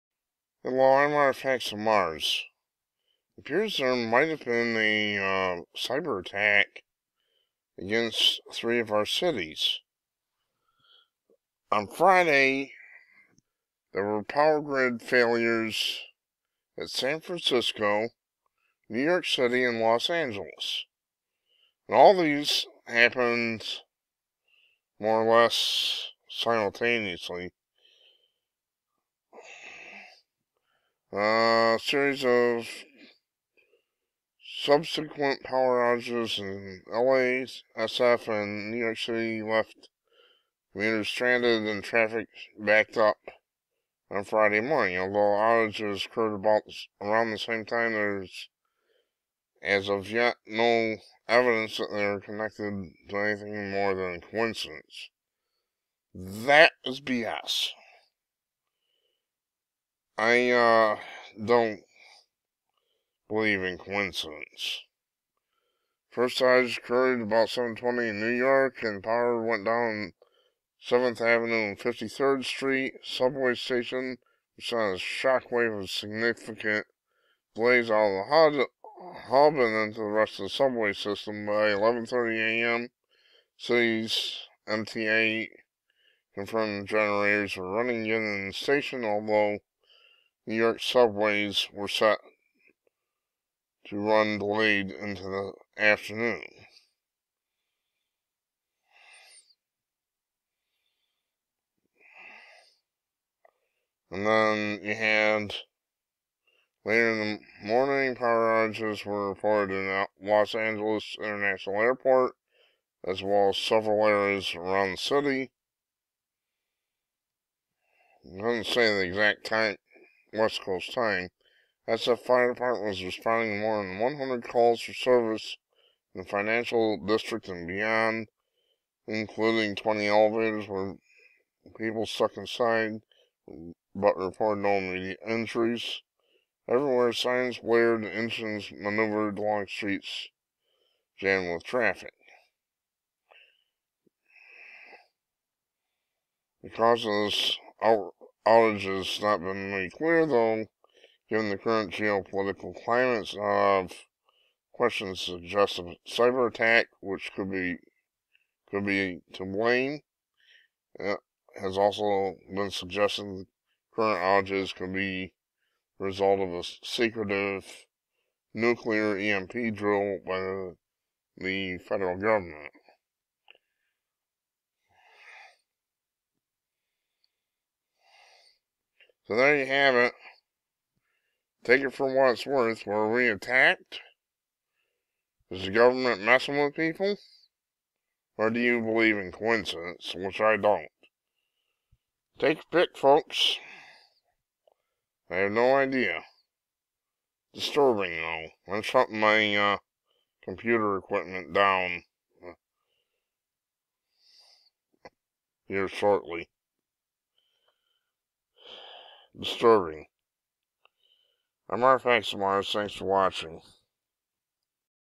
Three major cities hit by power outages, nearly at the same time. Cyber attack?
Los Angeles, NYC and San Francisco were struck by power outages almost at the same time on Friday, suggesting the possibility of a cyber attack or EMP test.